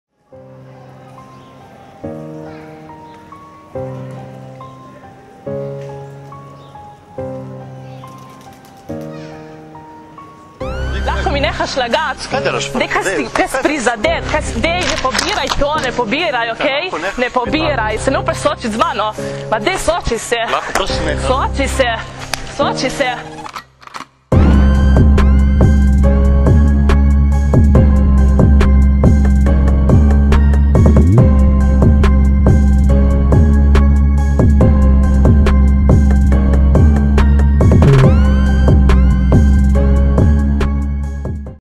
[TEASER VOL.2] Socialni eksperiment: Gledam, (ne) vidim
S tokratnim eksperimentom so, v okviru obeležitve mednarodnega dneva boja proti nasilju nad ženskami ŠOUP, PRIMSS, UŠZP in DNOBM, preverjali odzive posameznikov na nasilje v javnosti.